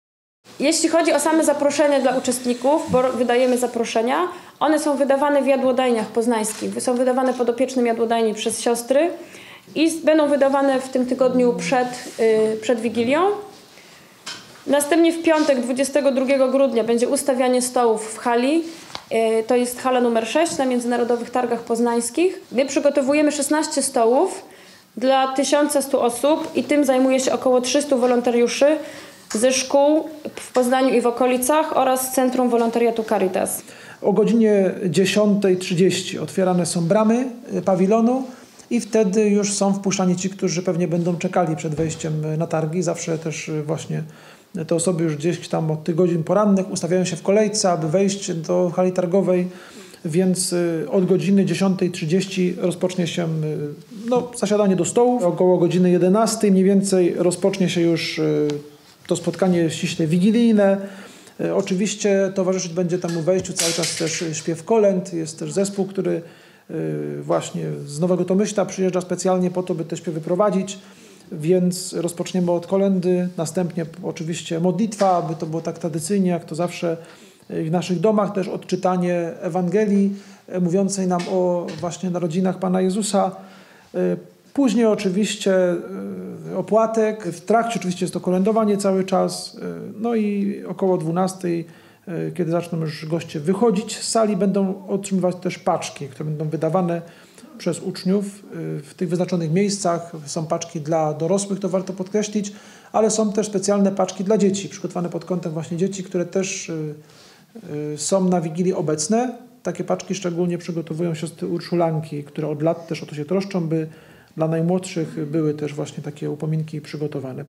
Caritas zorganizuje Wigilię dla potrzebujących
Tysiąc sto. Tyle miejsc oczekuje na potrzebujących podczas tegorocznej Wigilii organizowanej przez Caritas. Pomieści ich wszystkich hala nr 6 Międzynarodowych Targów Poznańskich. To gigantyczne przedsięwzięcie - trzeba przygotować nie tylko tysiące ciepłych posiłków, ale i paczek świątecznych.